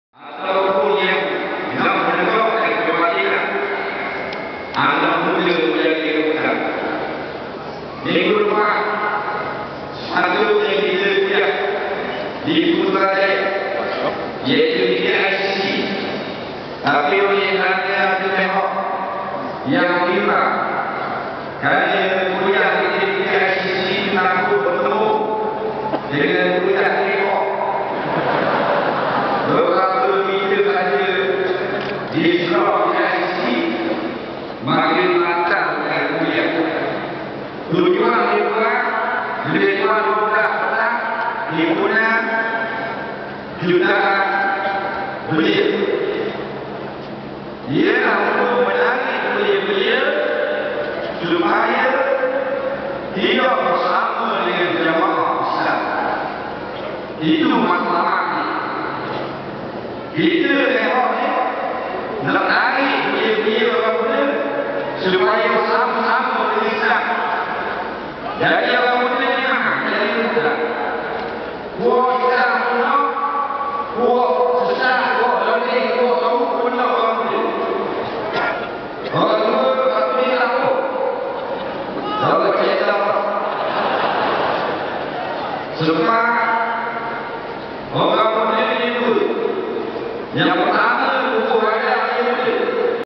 Sedutan Kuliah Asar Ustaz Azhar Idrus di Himpunan Hijau 1 Jun 2012
Sedutan daripada Kuliah Asar yang disampaikan oleh Ustaz Azhar Idrus di Himpunan Hijau Ke Putrajaya di Stadium Darul Aman, Alor Setar, Kedah pada 1 Jun 2012